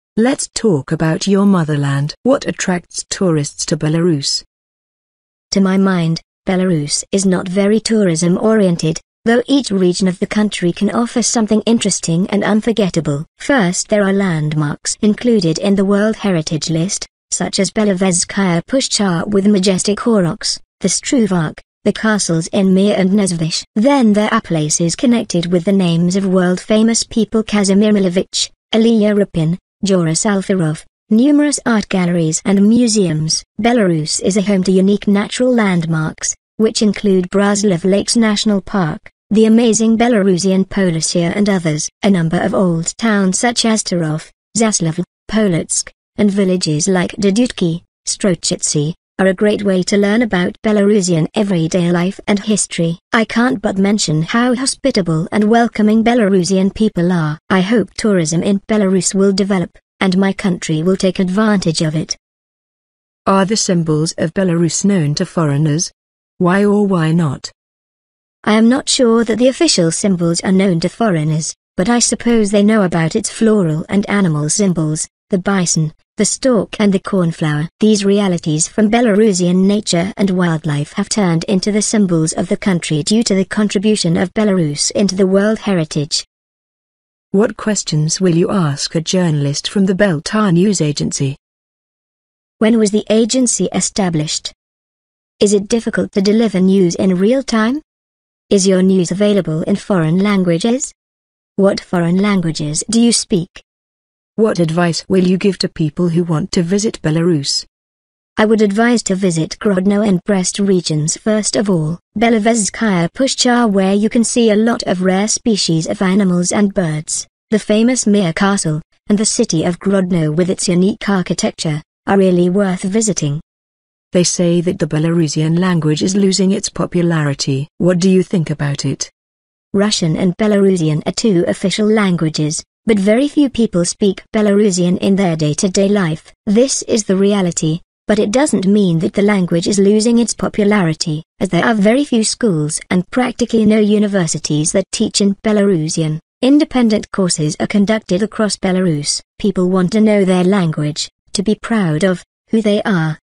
Билет 4
Let's talk about your Motherland. What attracts tourists to Belarus?